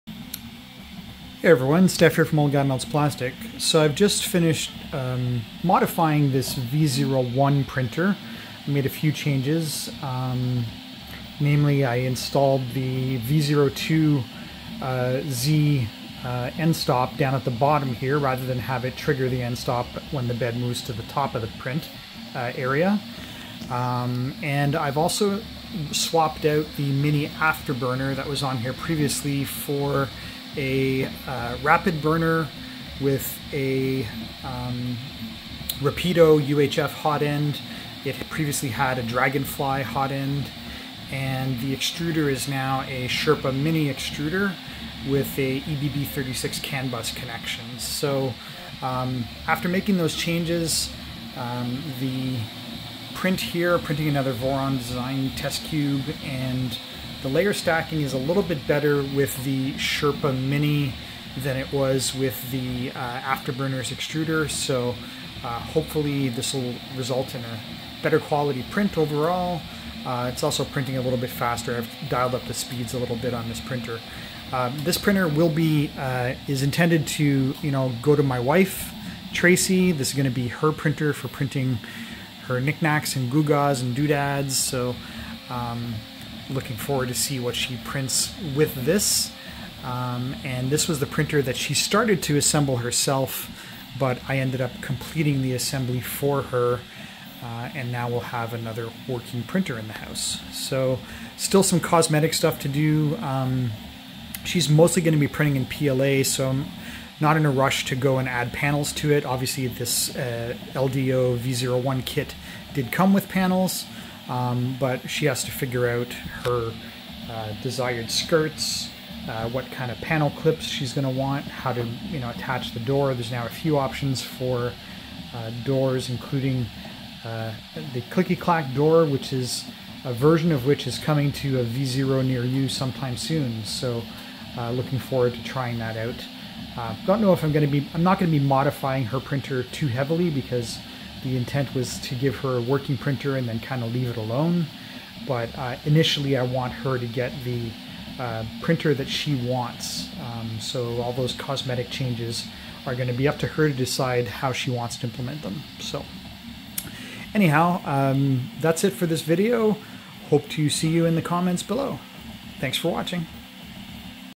My wife's V0.1 running a test print
Thanks to 3dlabtech.ca for selling me this LDO V0.1 a little over a year ago!

Tracy completed the assembly up until just before the point of installing the belts, and then this printer sat in her office collecting dust for the past year.

I finally convinced her to let me complete the build for her.